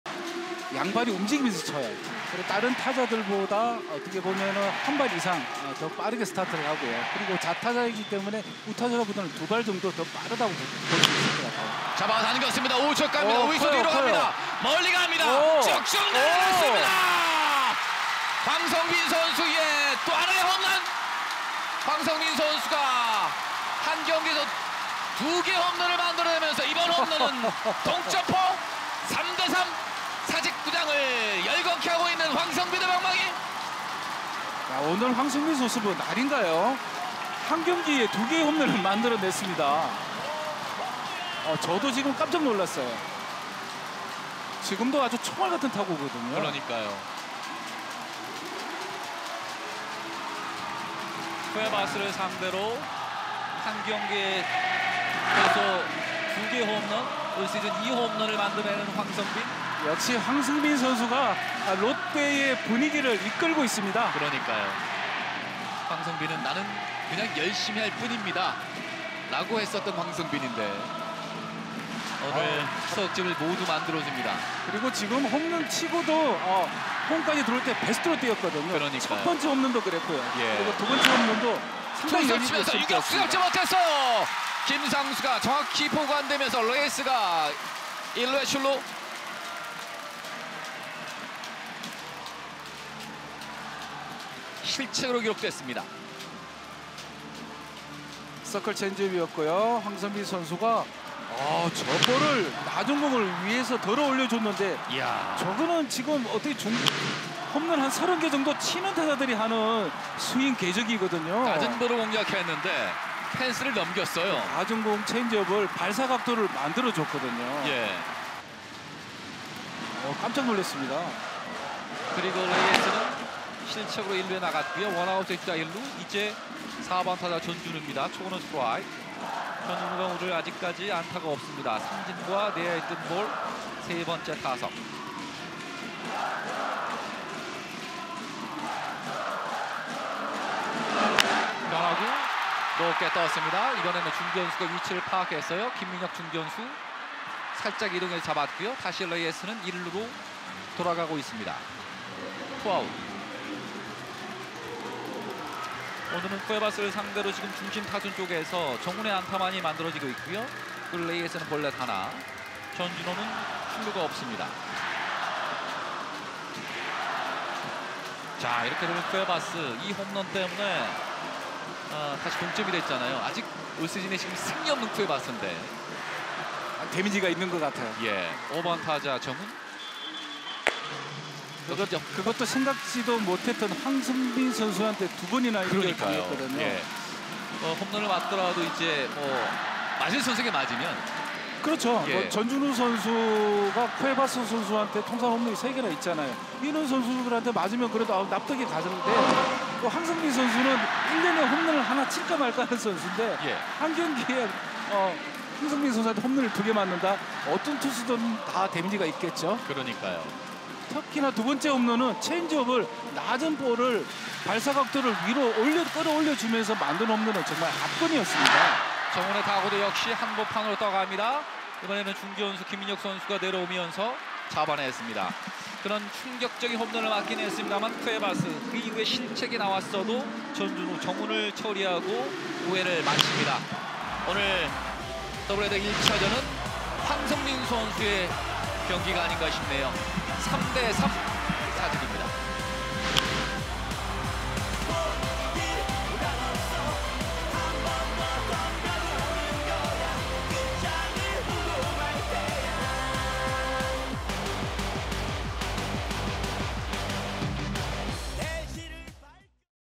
[KT vs 롯데_DH 1차전] KT 김상수를 감탄하게 만든 롯데 황성빈 1경기 2홈런!! | 4.21 | KBO 모먼트 | 야구 하이라이트
KBO 모먼트는 KBO 리그 야구 경기 중 주요 장면을 업로드합니다.
더 많은 KBO 모먼트를 원하시면 구독🔔 + 좋아요👍~!